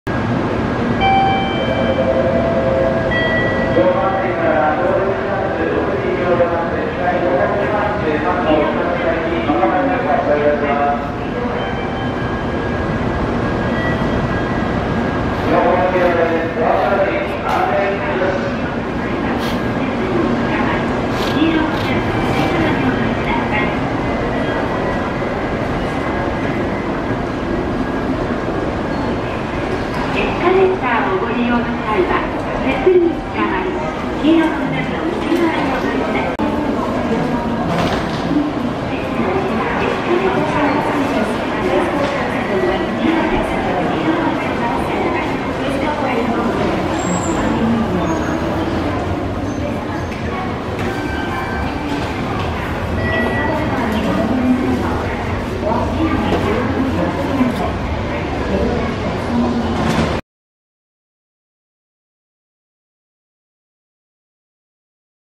【新横浜駅】新幹線から横浜線のりかえ
新幹線8号車から西のりかえ口を経由して横浜線へ行く方法です。